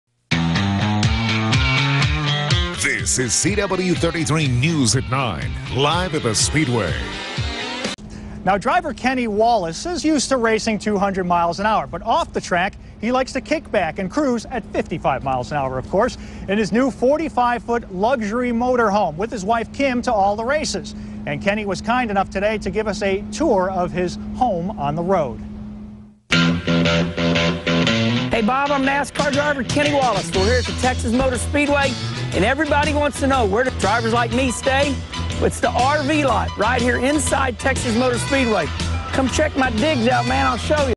Kenny Wallace RV- NASCAR
Looks like when Kenny's not doing 200 MPH, he's relaxing in his RV with his wife.  He has a sweet ride, on and off the track.